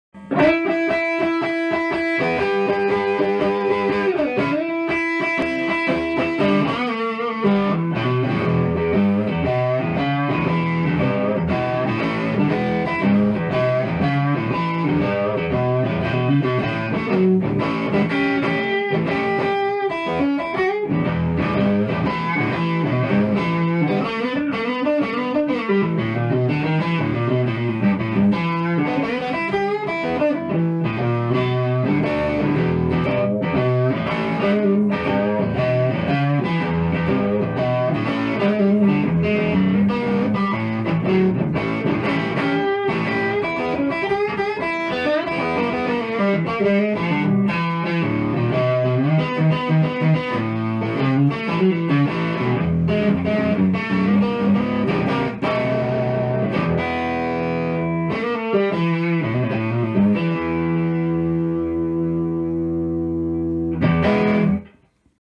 Pitcher Vorpal Guitar Amp Demo
Pitcher Vorpal 25 watt amp demo using a  Strat with no FX, SRV style